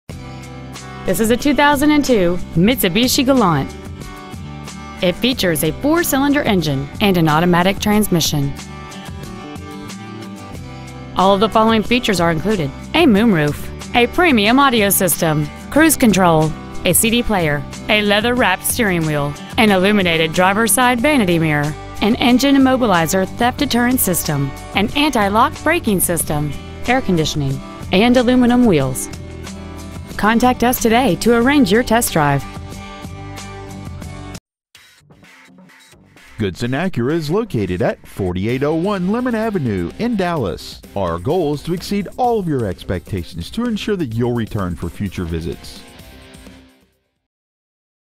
2002 Mitsubishi Galant Dallas TX
Year : 2002
 Make : Mitsubishi
 Model : Galant
 Engine : 4 Cyl - 2.40
 Trans . : Automatic
 Exterior : Sterling Silver Metallic
 Miles : 148,315
 Interior : Gray
 Stock : 2E038206

 Used 2002 Mitsubishi Galant Dallas TX